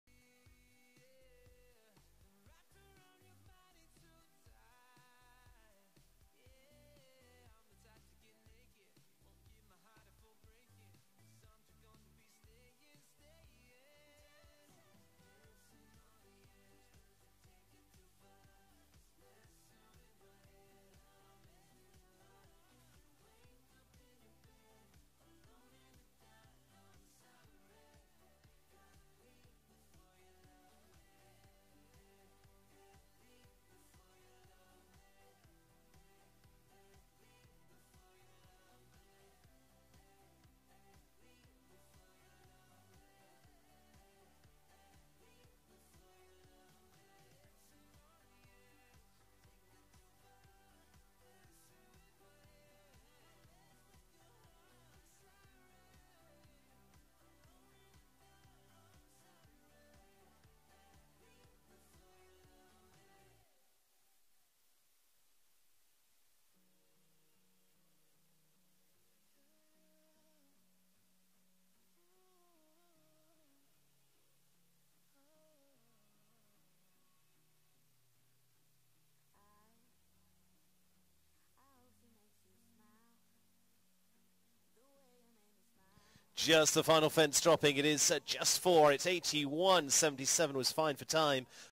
AMAZING II - WEF U25 Derby Field
WEF 2022
Wellington, Florida
"Amazing II" Owned by Mountain King Ranch, LLC, SWB Mare Ridden by Dominic Gibbs